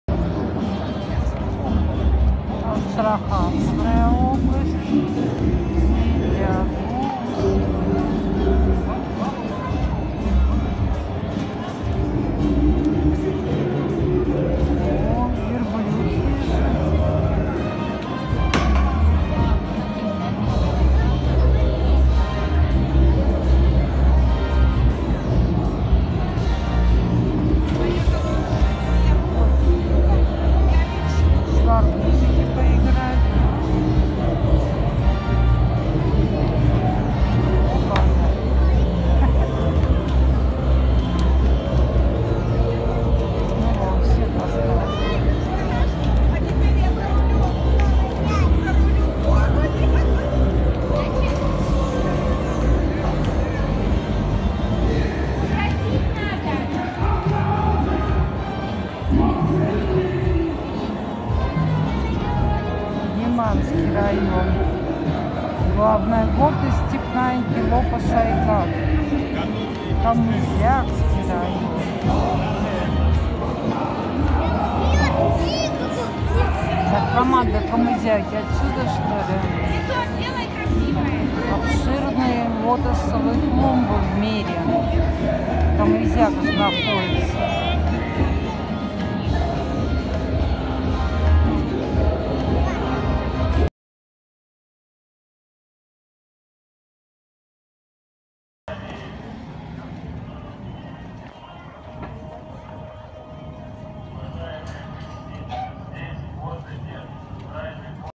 Астраханская область . Форум Россия 2023 . ВДНХ . Москва .
Субъект Российской Федерации, относится к Южному федеральному округу. Граничит с Волгоградской областью — на севере, с Калмыкией — на западе, с Казахстаном — на востоке, на юге — омывается Каспийским морем. Административный центр области — город Астрахань. Область образована 27 декабря 1943 года.